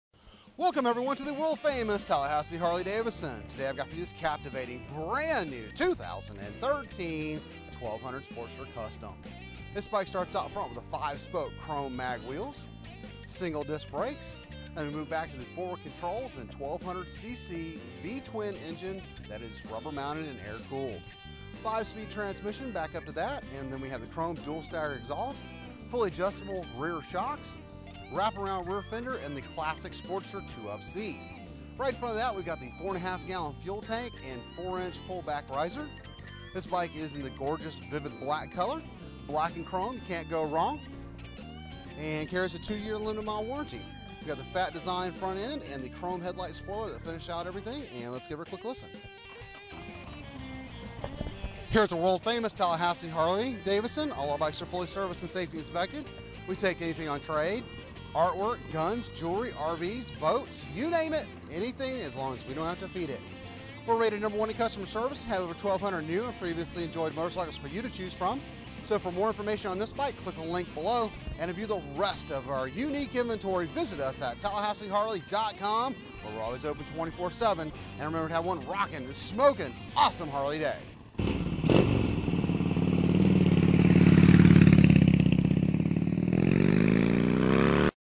New 2013 Harley Davidson 1200 Custom Motorcycle for sale
New 2013 Harley Davidson XL1200C Sportster 1200 Custom Motorcycles for sale.
Featuring: Vivid Black

Stock# HD5975